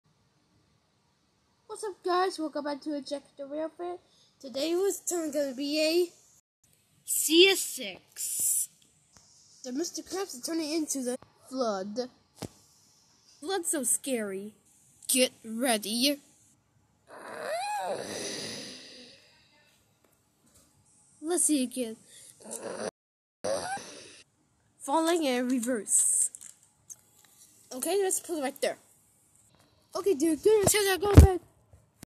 hurricane train derails in Montgomery AL
CSX train going down to the water bridge are broken on CSX M&M subvisoins